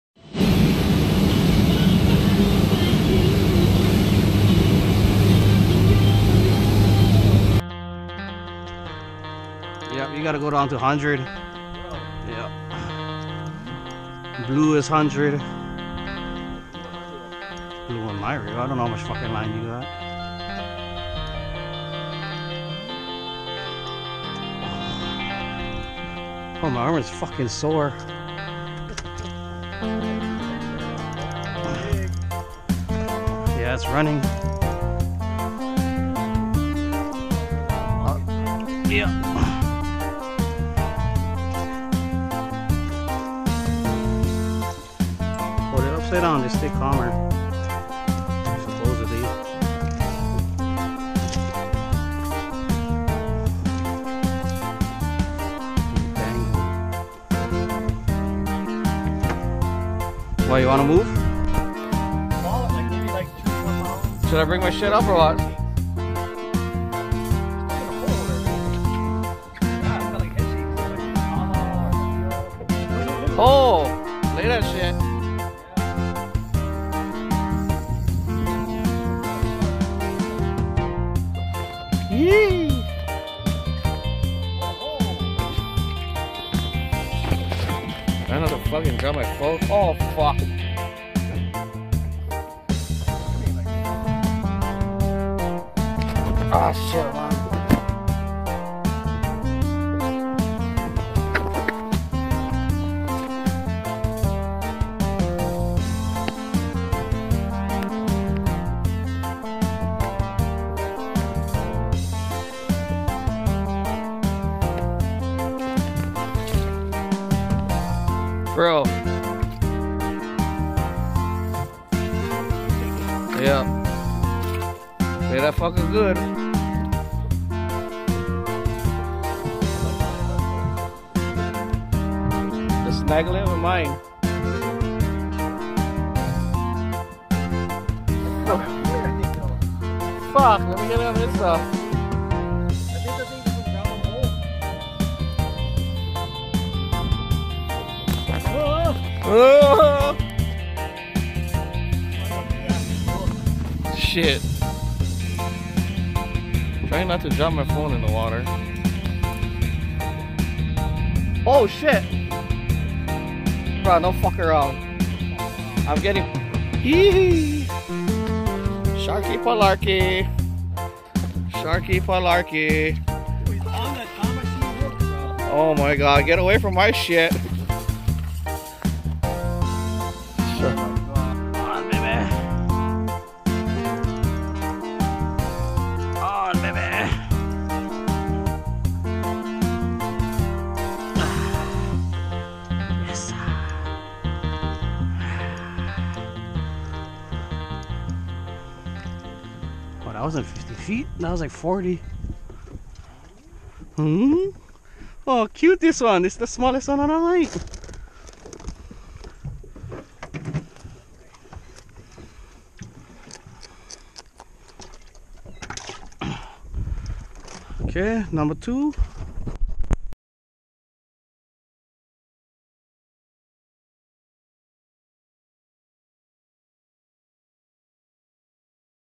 fast kine akule mission.. aka big eye scad . plus sharkyd footage lol
fast kine akule mission.

gear used
ulua junkies gear
teramar 9ft
stradic 5k 30lb braid.

Mines
shimano nexave 5k
daiwa coastal 9ft

then switched to a okuma rock away 10ft
daiwa bg 4500